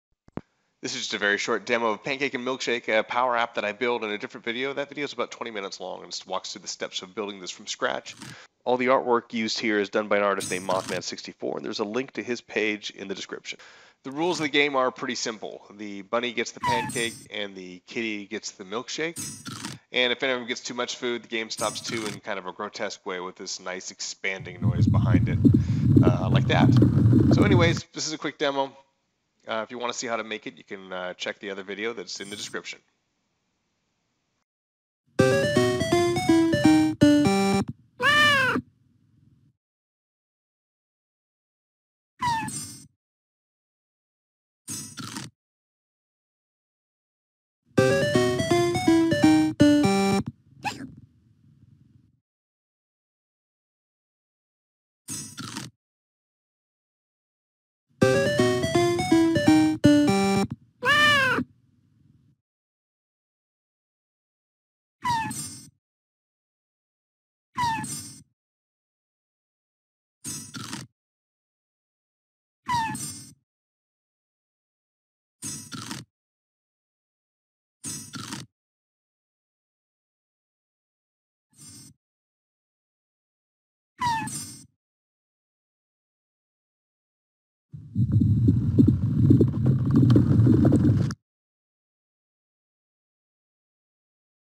Pancake & Milkshake - PowerApp Demo
Pancake & Milkshake - The rules are simple: The kitty gets the milkshake…the bunny gets the pancake.  This is a short demo of an app inspired by the Wreck It Ralph 2 trailer (check it out, it's hilarious)